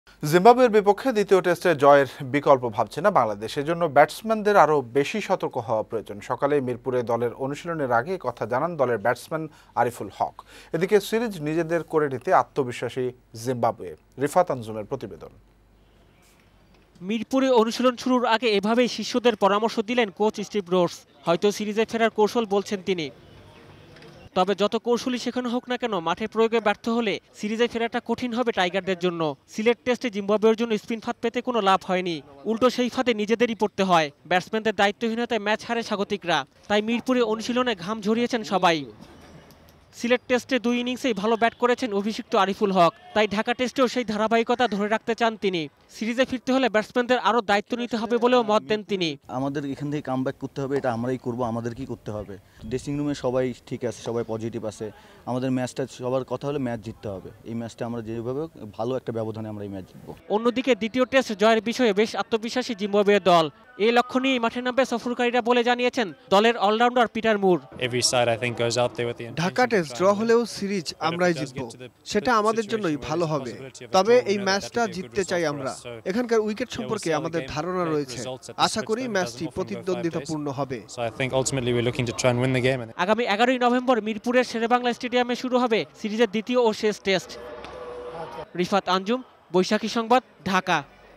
জিম্বাবুয়ের বিপক্ষে দ্বিতীয় টেস্টে জয় পেতে মরিয়া বাংলাদেশ | Cricket | News | Rifat | 09Nov18
জিম্বাবুয়ের বিপক্ষে দ্বিতীয় টেস্টে জয় পেতে মরিয়া বাংলাদেশ, মিরপুরে অনুশীলন;  সিরিজ জয়ের বিকল্প ভাবছে না সফরকারীরা।